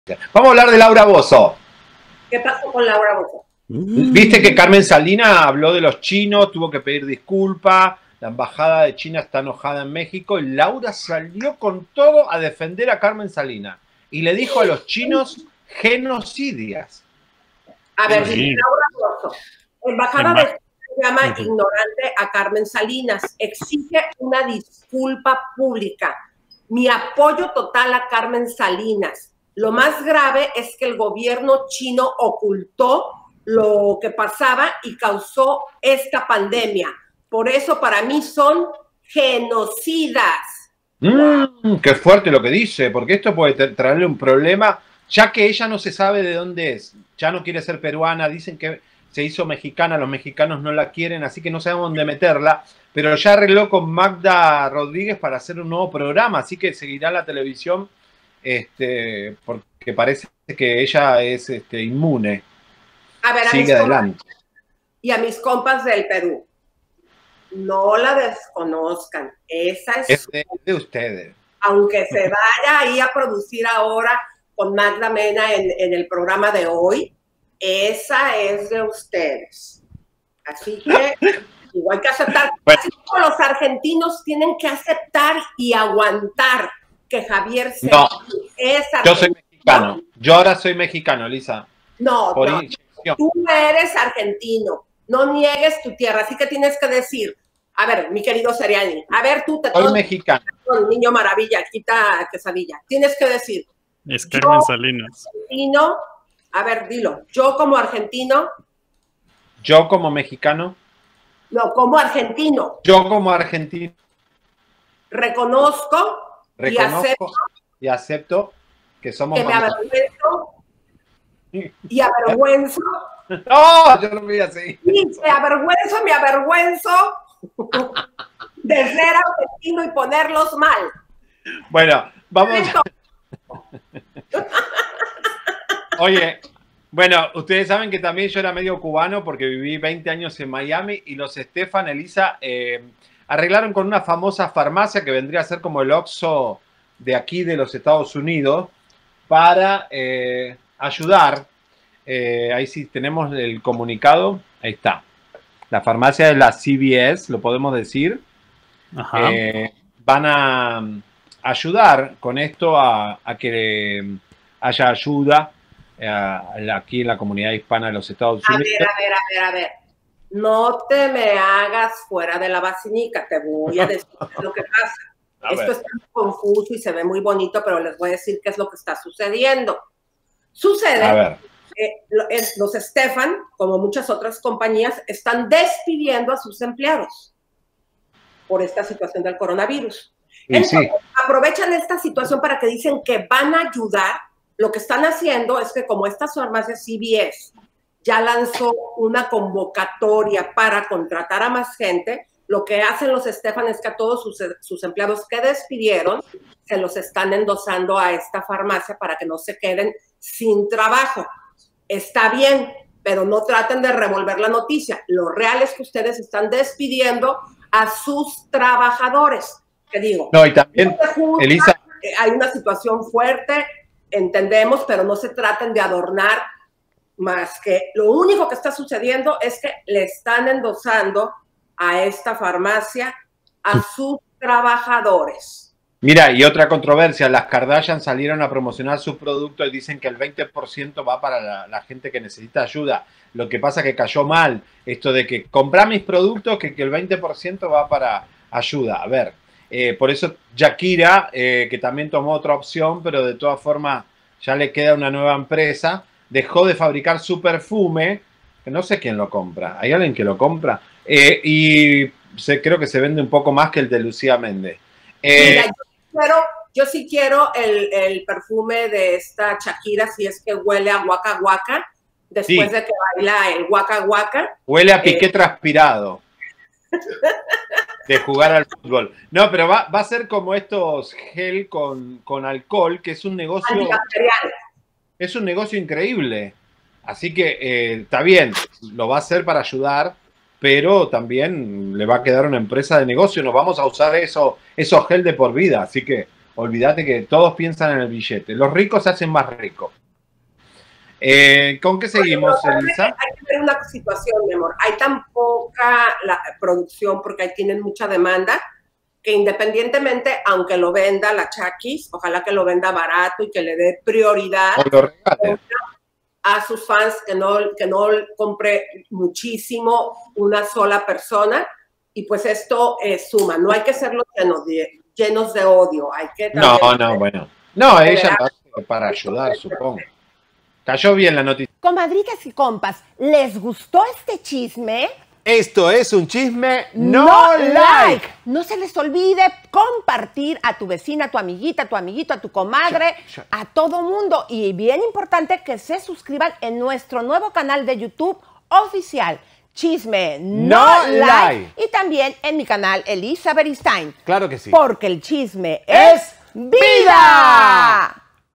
LAURA BOZZO APOYA LOS COMENTARIOS IGNORANTES DE CARMEN SALINAS ACERCA DE CHINA - CHISME NO LIKE
Carmen Salinas tuvo que pedir disculpas a China por decirles Chinitos Come Perros y Gatos. La sorpresa ahora es que Laura Bozzo la apoya incondicionalmente por ser comadres argüenderas de lo “pior”.